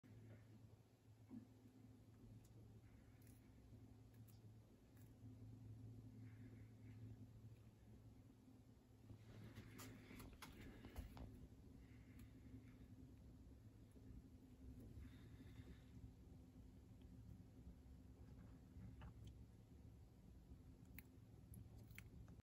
亀たちの朝ごはん
7時くらいの様子。朝から元気です。